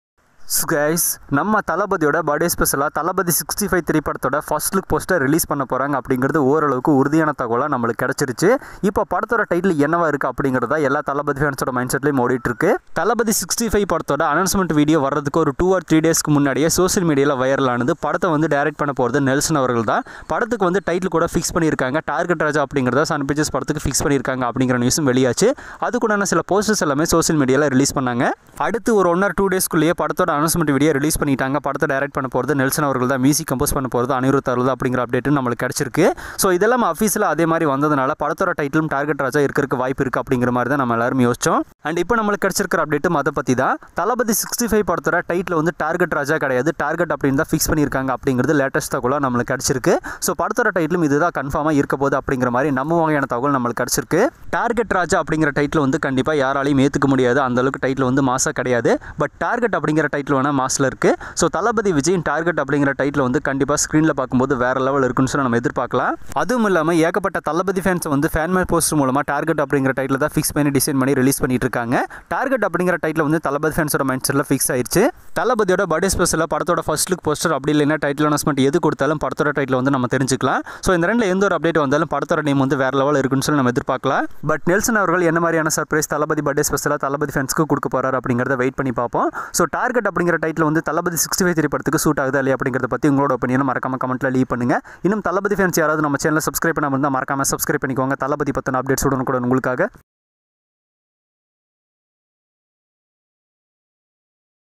Breaking : Thalapathy 65 title Update | June 21st Thalapathy birthday Special | Sun Pictures
Finally Thalapathy 65 title TARGET , Sun pictures official from June 21st.!

Thalapathy vijay cinema updates and all news uploading in